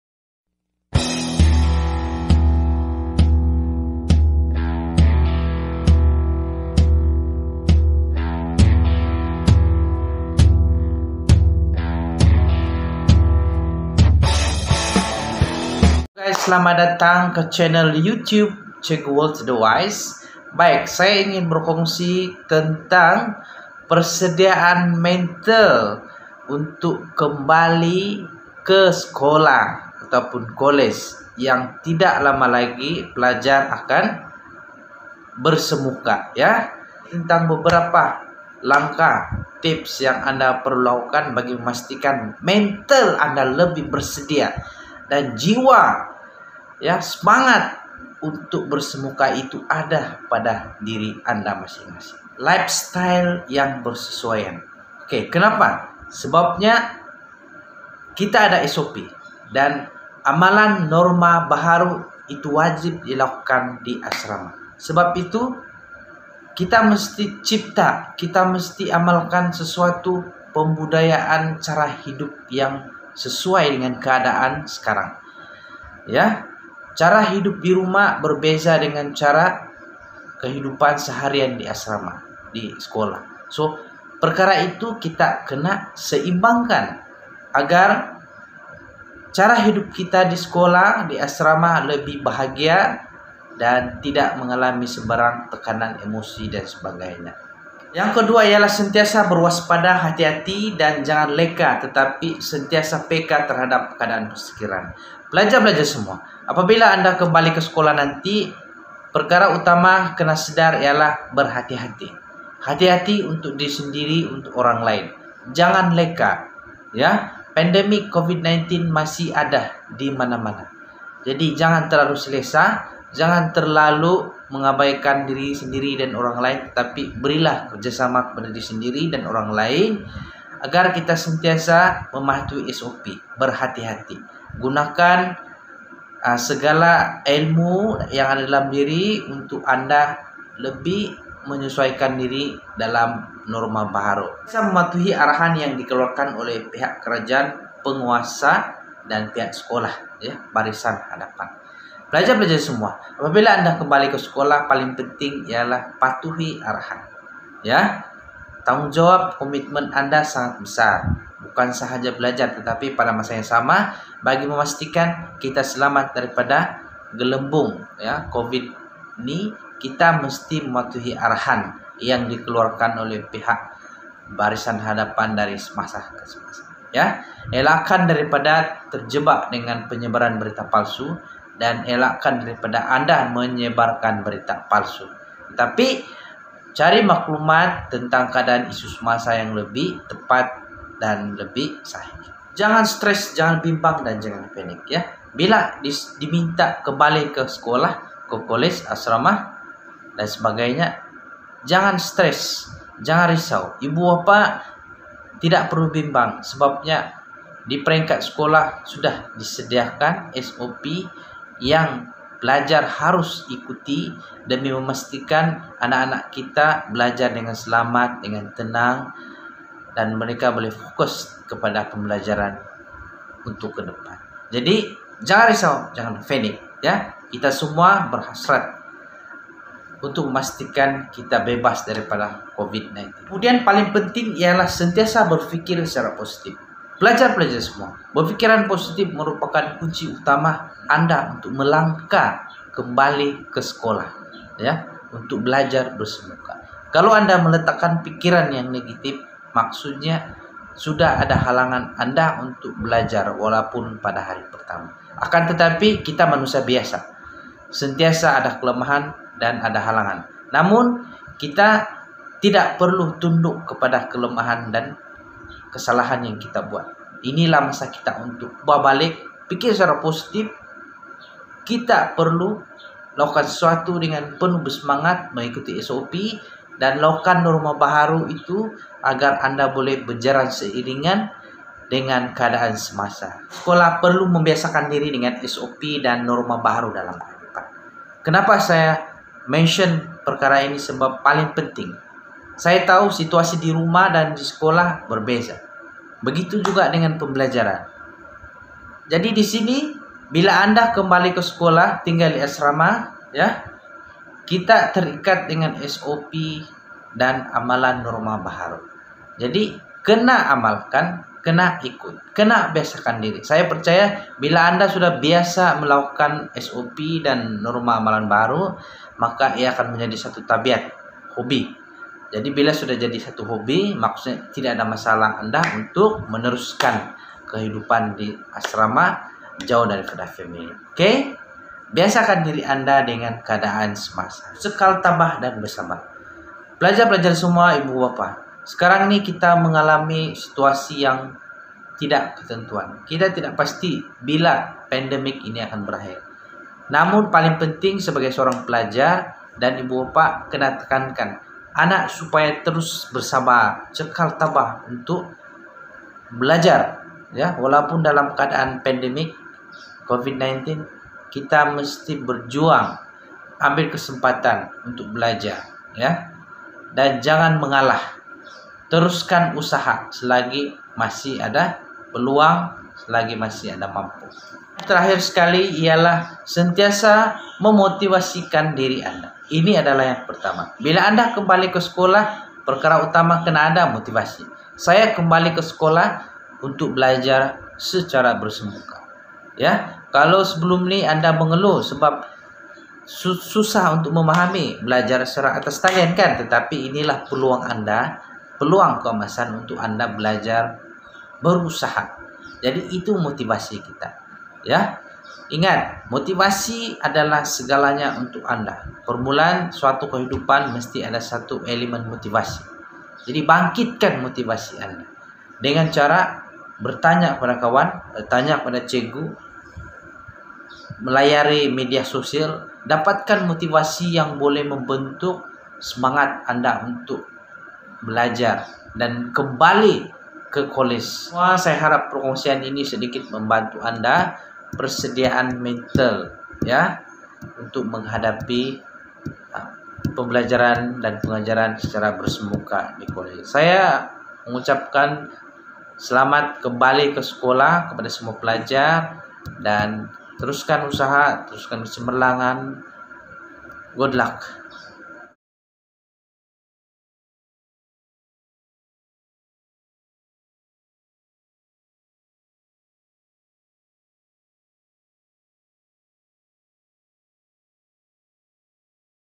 PERSEDIAAN MENTAL UNTUK KEMBALI KE SEKOLAH DI MUSIM PANDEMIK COVID 19
Persediaan Mental sangat penting untuk pelajar bekalkan apabila berada di asrama, kolej atau sekolah untuk menjalankan aktiviti pembelajaran dan pengajaran secara bersemuka.